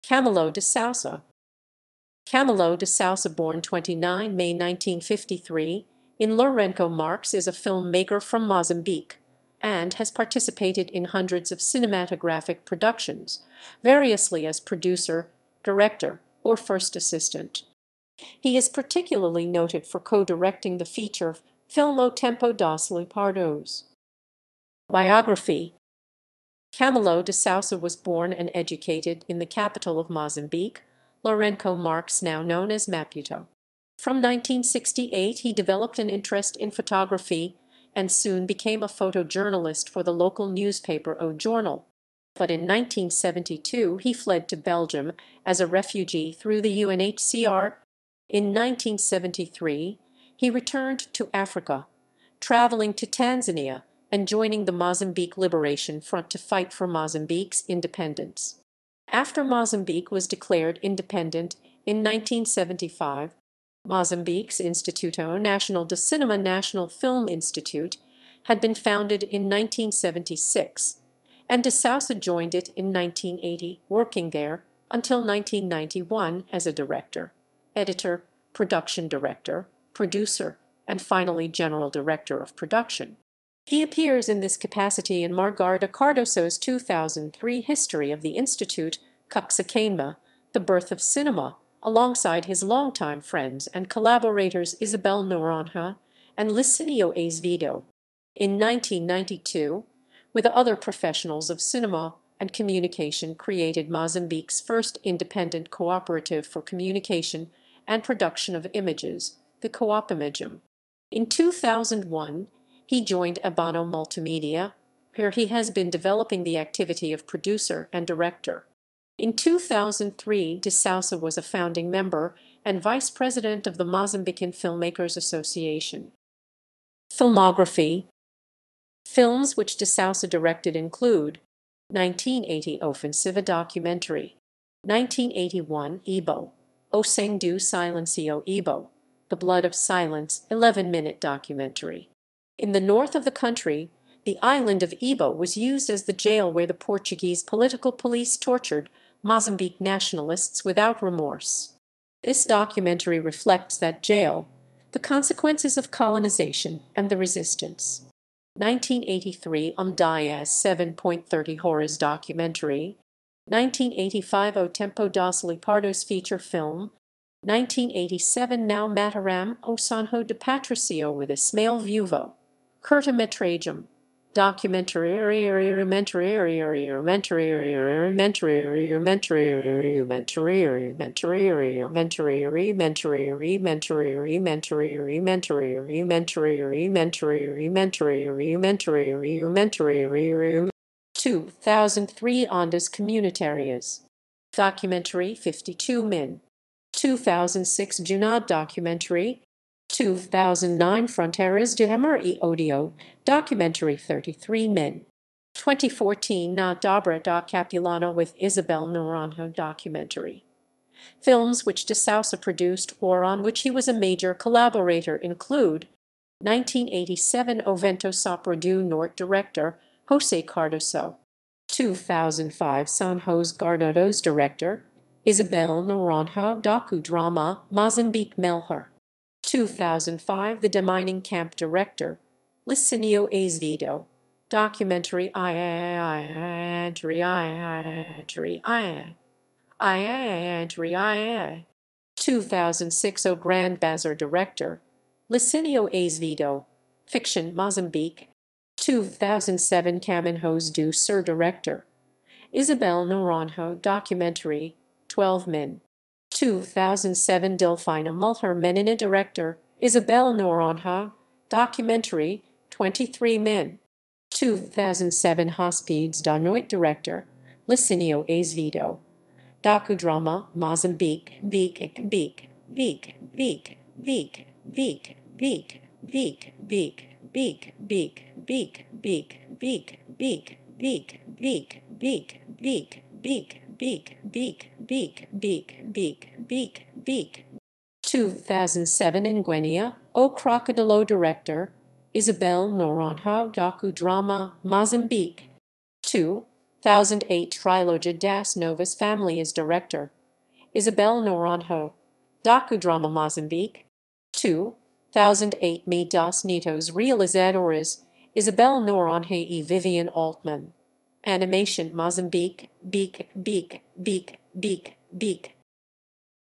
Camilo de Sousa
Camilo de Sousa (born 29 May 1953 in Lourenço Marques) is a film-maker from Mozambique, and has participated in hundreds of cinematographic productions, variously as producer, director, or first assistant. He is particularly noted for co-directing the feature film O Tempo dos Leopardos.